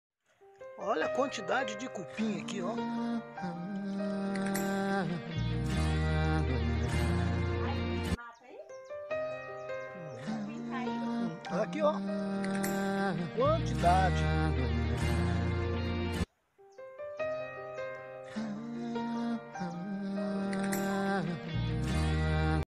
nome científico é Cryptotermes brevis. - CUPIM ( março 2022)
Quais são as características do cupim de madeira seca? O cupim de madeira seca necessita de ambientes com pouca umidade para se desenvolver. Diferente do cupim subterrâneo, a sua colônia fica dentro do objeto infestado. Geralmente abriga-se em estruturas e móveis de madeira.